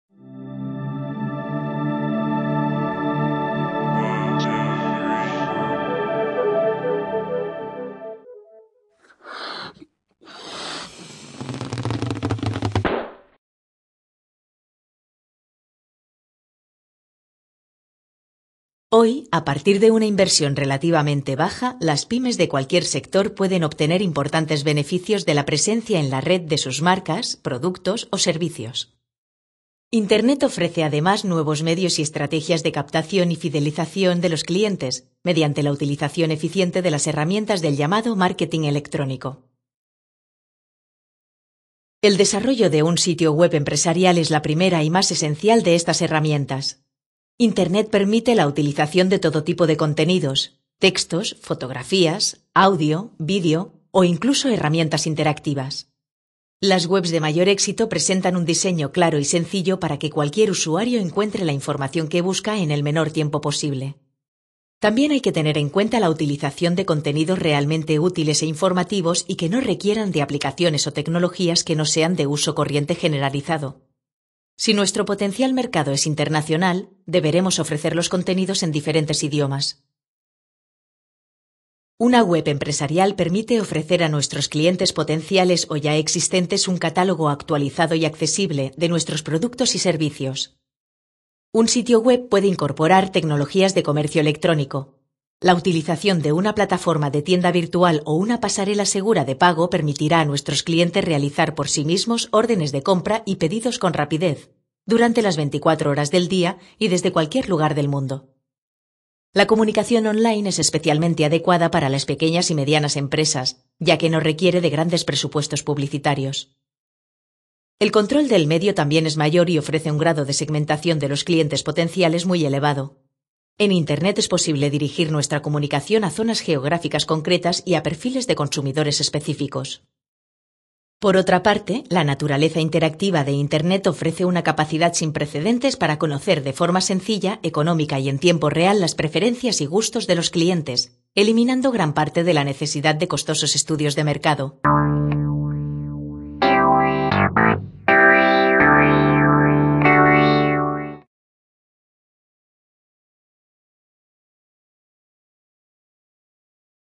Herramientas de márqueting electrónico. Presencia en la Red I
Internet se ha convertido en una herramienta fundamental para las decisiones de compra de un creciente número de consumidores y empresas. Hoy, a partir de una inversión relativamente baja, las pymes de cualquier sector pueden obtener importantes beneficios de la presencia en la Red de sus marcas, productos o servicios. Las nuevas tecnologias ofrecen además nuevos medios y estrategias de captación y fidelización de los clientes mediante la utilización eficiente de las herramientas del llamado marketing electrónico. En este primer clip se tratan los temas de visualización, usabilidad, catálogo On Line o comercio eletrónico, entre otros.